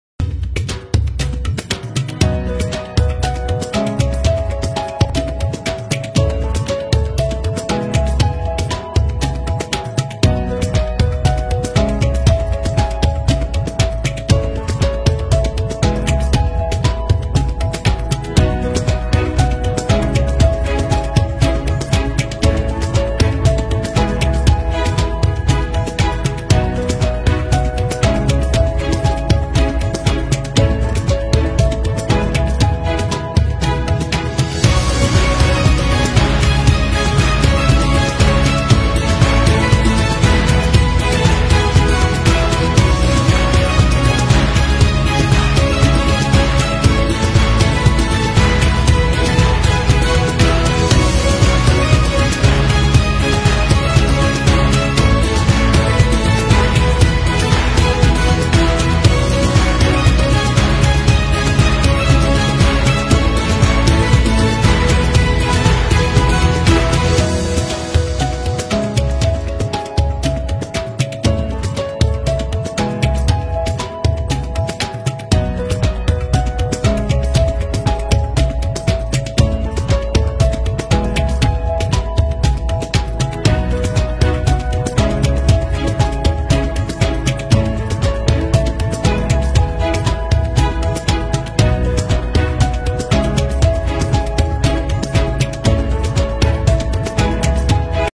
75th INDEPENDENCE DAY.
Kuwait painted 100 buses in the Tri colour on the occasion of our Independence Day.
Jay ho.